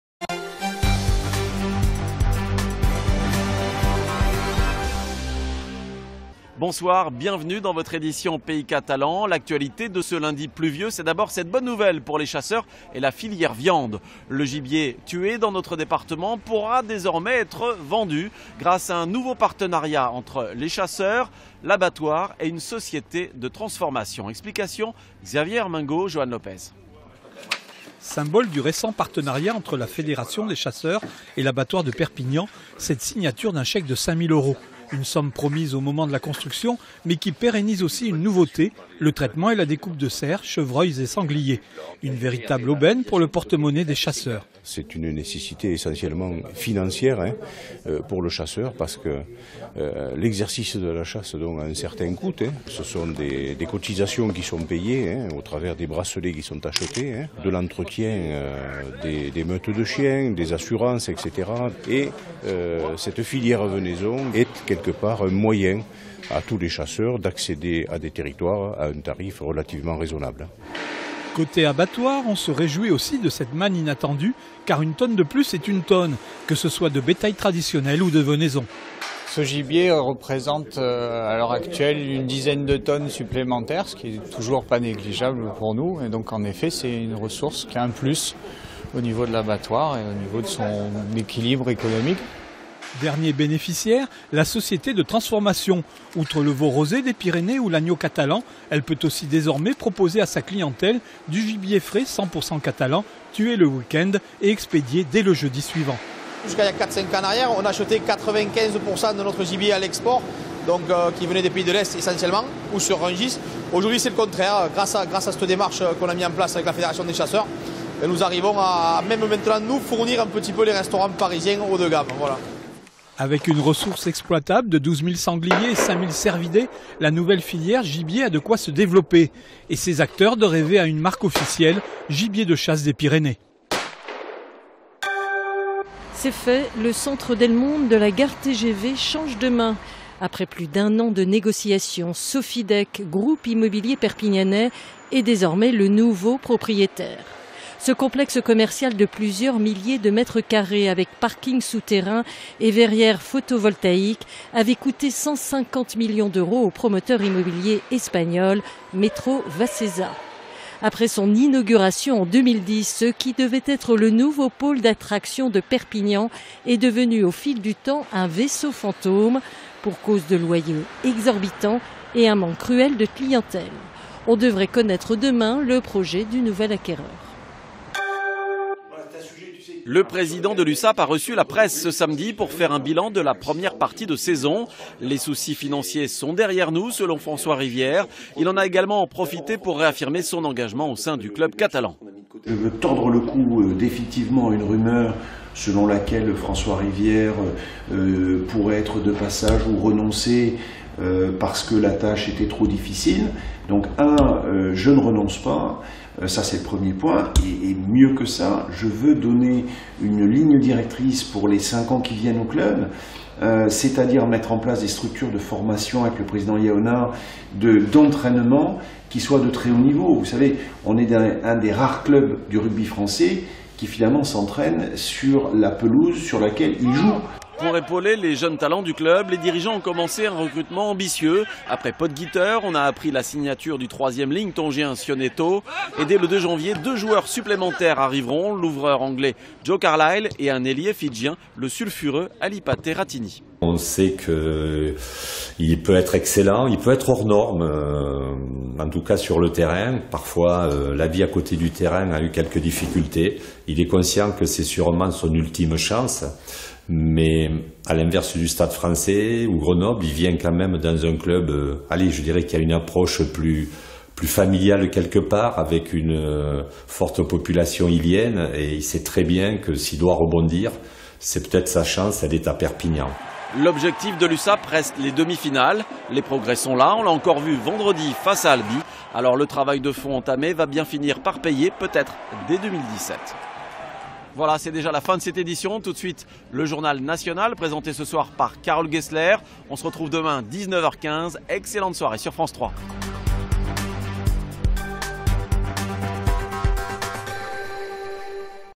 France 3 PaysCatalan- reportage sur la filière venaison
La FDC66 a initié un partenariat avec les établissements Guash et l'abattoir de Perpignan pour créer une véritable filière locale de traitement de la venaison.